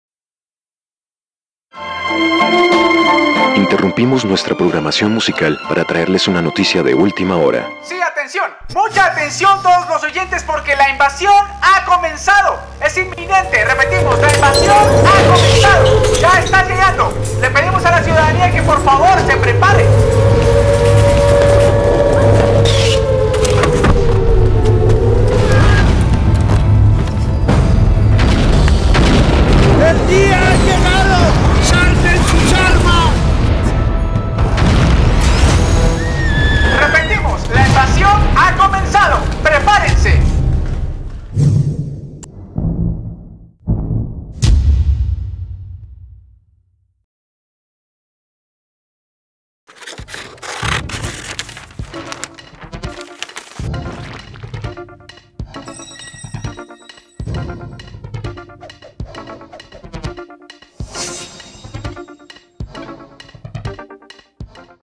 IMAGINATON 2012 / 2013 - Promo La Invasión ha Comenzado y Armado Robot
Hasta el 31 de agosto hay oportunidad para participar en IMAGINATON, V Maratón Nacional de Realización y Exhibición Audiovisual.
Esta promo, referencia LA INVASIÓN HA COMENZADO, es una producción de COPETE para Imaginaton 2012.